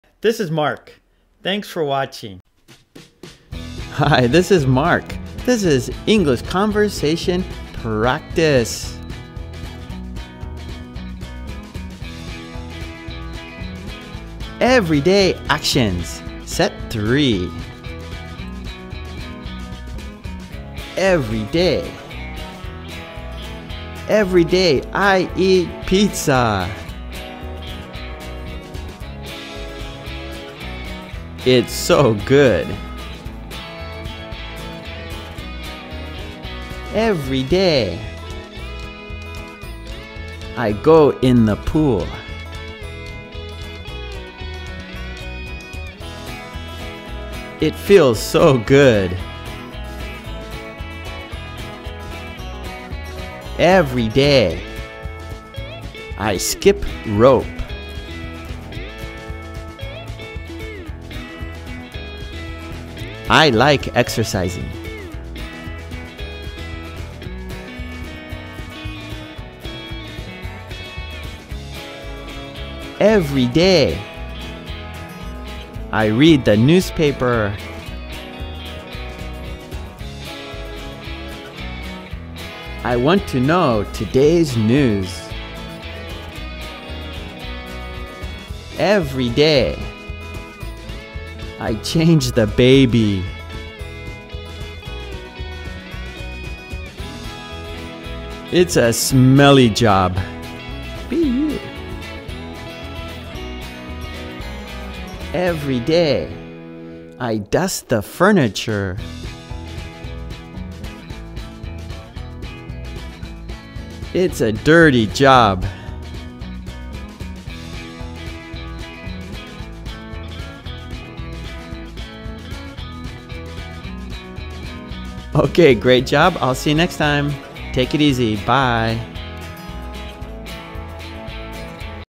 Learn Everyday Actions. Set 3. Easy English Conversation Practice. ESL.
Study things people do every day.
Speak English Now!